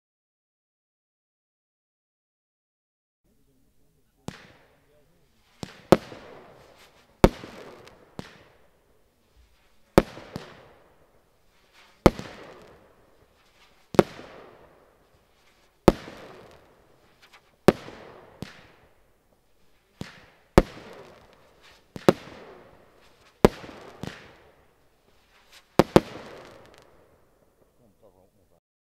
01229 The beast - Zena Vuurwerk [OFFICIAL VIDEO]
Deze Zena cake schiet 12 dikke gouden blink boekketten met spinnende tails de lucht in. Zulke spinnende staarten heb je nog nooit gezien.